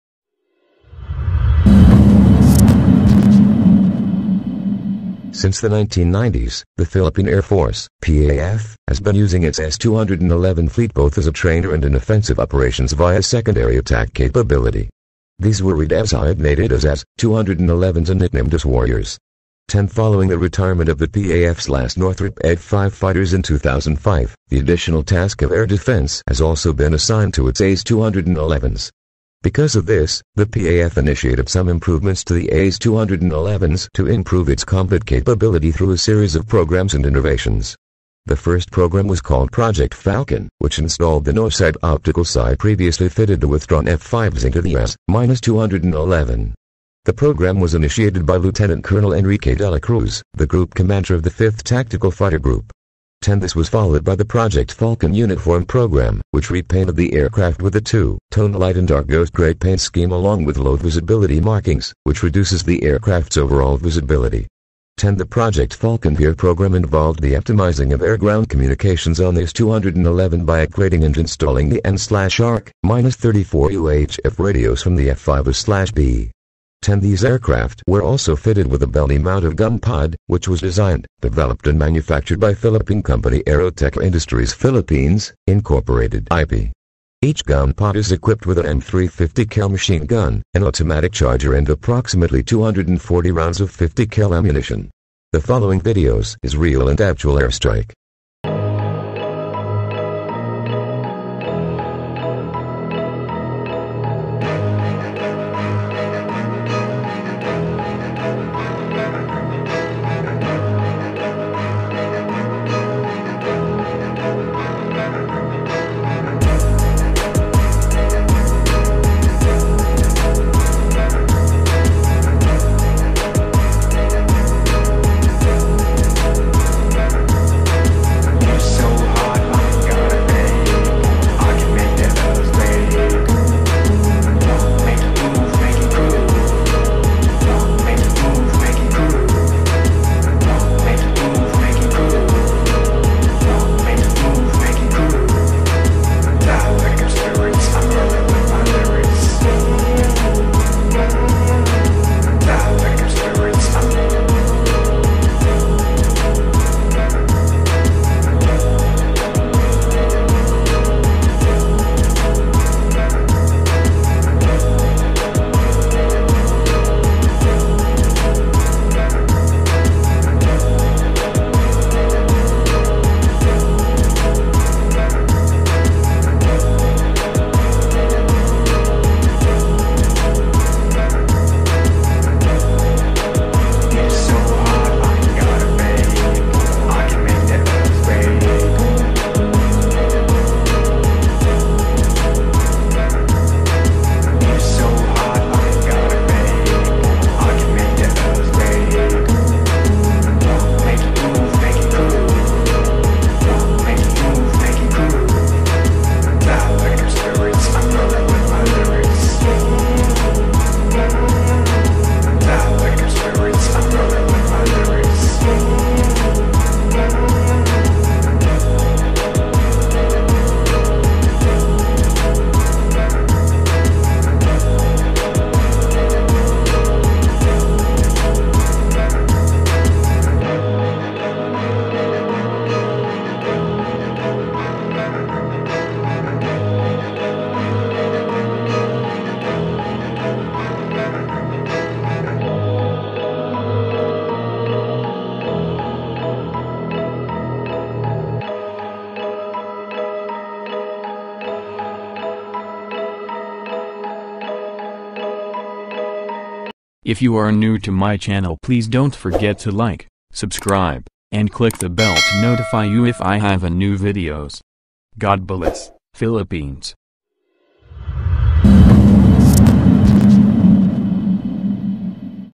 FOOTAGE OF PHILIPPINE AIR FORCE S-211 AIRSTRIKE! Actual and Real Airstrike!
This video is 10years ago.

Since the 1990s, the Philippine Air Force (PAF) has been using its S-211 fleet both as a trainer and in offensive operations via secondary attack capability. These were redesignated as AS-211s and nicknamed as "Warriors".[10] Following the retirement of the PAF's last Northrop F-5 fighters in 2005, the additional task of air defense has also been assigned to its AS-211s. Because of this, the PAF initiated some improvements to the AS-211s to improve its combat capability through a series of programs and innovations.

The first program was called "Project Falcon", which installed the Norsight Optical Sight previously fitted to withdrawn F-5s into the AS-211. The program was initiated by Lieutenant Colonel Enrique Dela Cruz, the Group Commander of the 5th Tactical Fighter Group.[10] This was followed by the "Project Falcon Uniform" program, which repainted the aircraft with a 2-tone light-and-dark ghost gray paint scheme along with low visibility markings, which reduces the aircraft's overall visibility.[10]

The "Project Falcon Hear" program involved the optimizing of air-ground communications on the AS-211 by upgrading and installing the AN/ARC-34 UHF radios from the F-5A/B.[10] These aircraft were also fitted with a belly-mounted gun pod, which was designed, developed and manufactured by Philippine company Aerotech Industries Philippines, Inc. (AIPI). Each gun pod is equipped with an M3 .50 cal machine gun, an automatic charger and approximately 240 rounds of .50 cal ammunition.

We do not claim the photo and video,we use this for news reporting purpose only